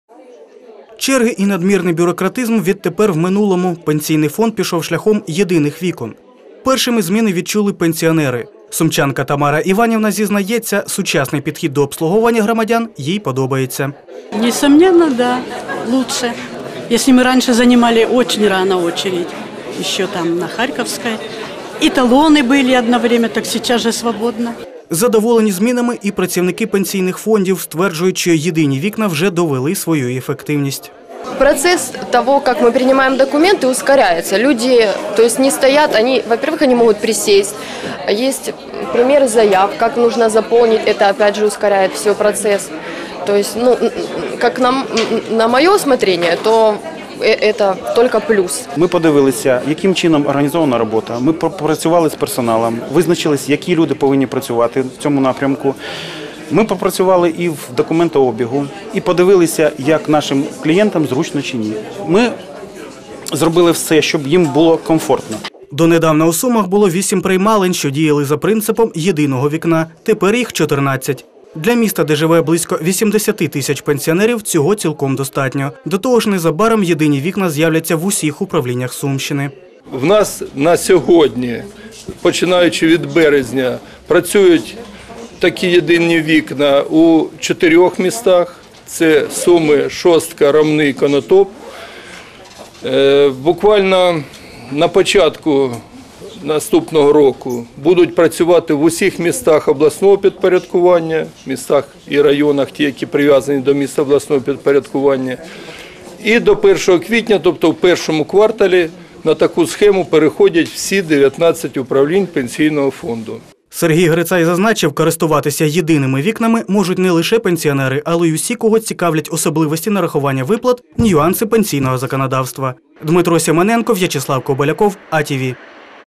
За принципом «єдиного вікна» працюватиме пенсійний фонд
За принципом «єдиного вікна» працюватиме пенсійний фонд на Сумщині. Про це повідомив його очільник Сергій Грицай.  «Єдині вікна» вже працюють у чотирьох містах області і довели свою ефективність.